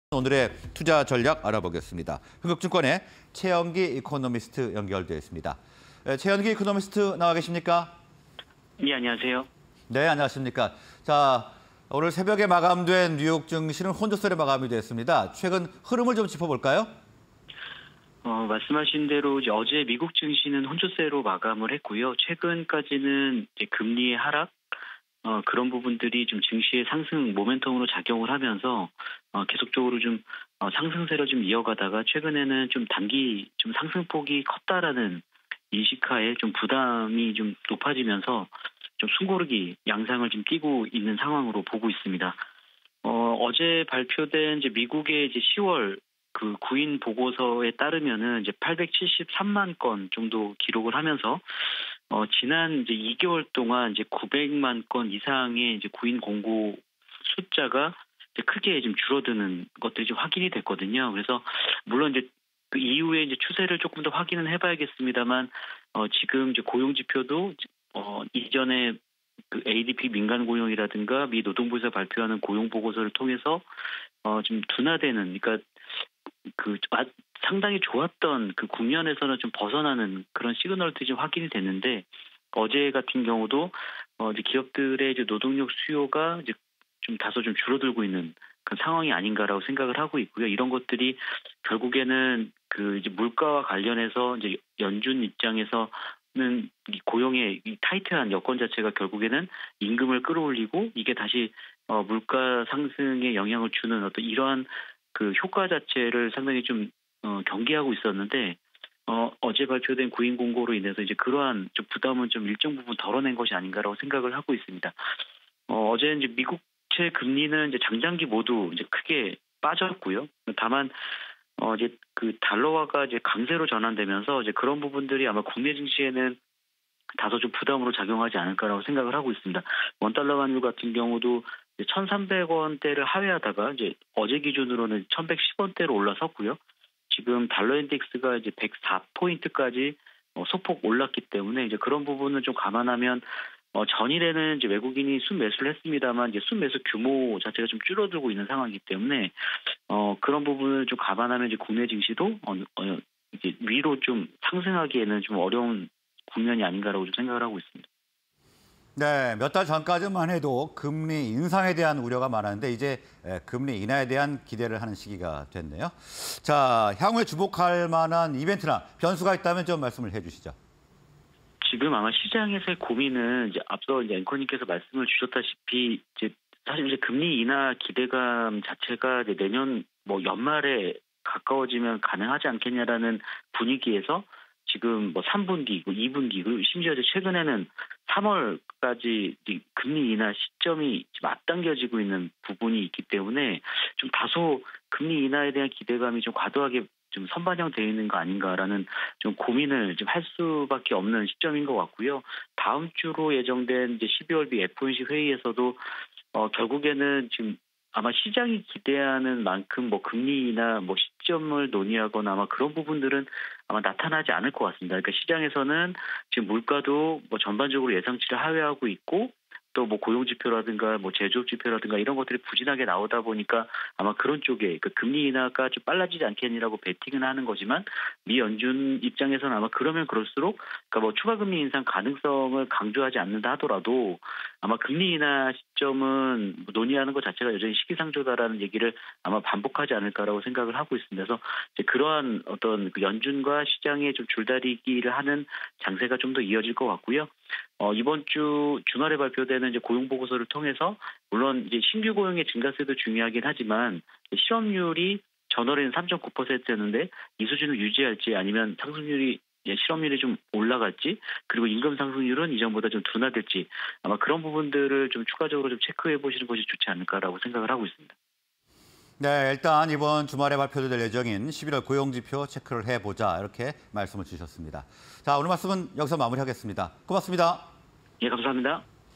촉매 부족한 美증시, 숨고르기 진입하나? / 금리 인하 기대감 되돌리나… 주목할 변수는?_투자 인사이트 (20231206)
Q. 촉매 부족한 美증시, 숨고르기 진입하나?
Q. 금리 인하 기대감 되돌리나… 주목할 변수는?